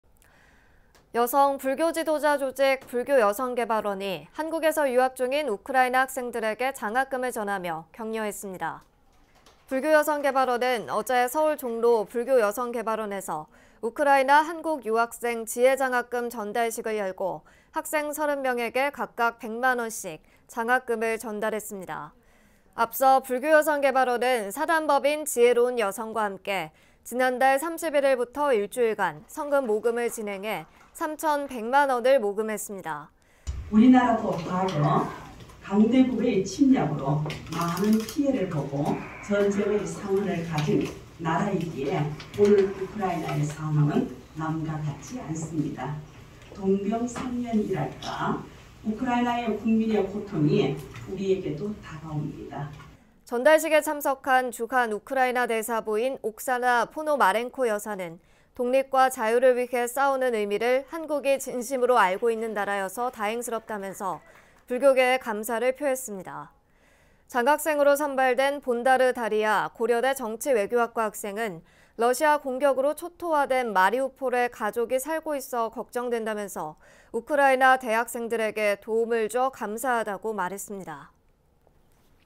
[BBS뉴스] 불교여성개발원 우크라이나 한국유학생 장학금 전달식
여성 불교 지도자 조직 '불교여성개발원'이 한국에서 유학 중인 우크라이나 학생들에게 장학금을 전하며 격려했습니다.

불교여성개발원은 어제(8일) 서울 종로 불교여성개발원에서 '우크라이나 한국 유학생 지혜장학금 전달식'을 열고 학생 30명에게 각각 100만 원씩 장학금을 전달했습니다.

앞서 불교여성개발원은 사단법인 '지혜로운여성'과 함께 지난달 31일부터 일주일간 성금 모금을 진행해 3천100만 원을 모금했습니다.

[김선옥 / 불교여성개발원 원장]
["우리나라 또한 강대국의 침략으로 많은 피해를 보고 전쟁의 상흔을 가진 나라이기에 오늘 우크라이나의 상황은 남일 같지 않습니다. 동병상련이랄까. 우크라이나 국민의 고통이 우리에게도 다가옵니다."]

전달식에 참석한 주한 우크라이나 대사 부인 옥사나 포노마렌코 여사는 "독립과 자유를 위해 싸우는 의미를 한국이 진심으로 알고 있는 나라여서 다행스럽다"면서 불교계에 감사를 표했습니다.

장학생으로 선발된 본다르 다리아 고려대 정치외교학과 학생은 "러시아 공격으로 초토화된 마리우폴에 가족이 살고 있어 걱정된다"면서 "우크라이나 대학생들에게 도움을 줘 감사하다"고 말했습니다.